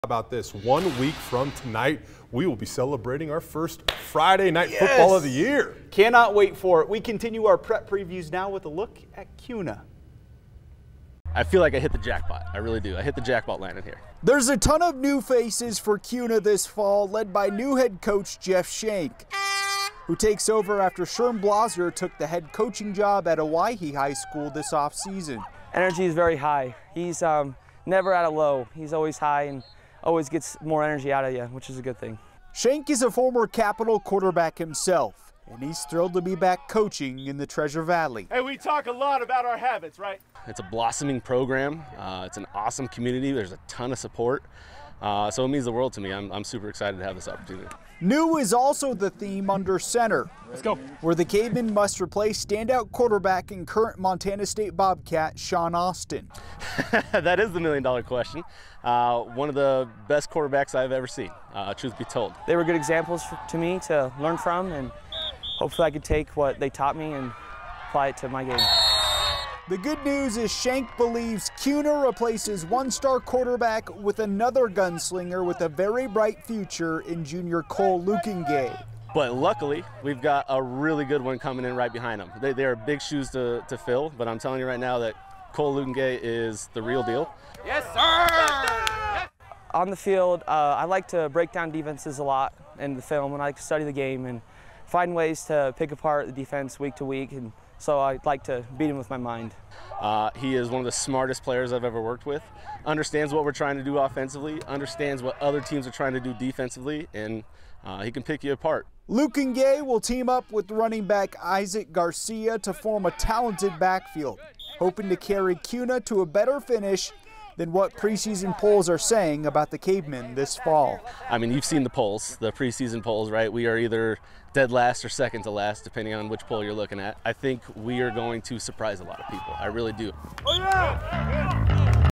Friday Night Football preview: Kuna Kavemen football
There's a ton of new faces for Kuna football this year led by a new head coach.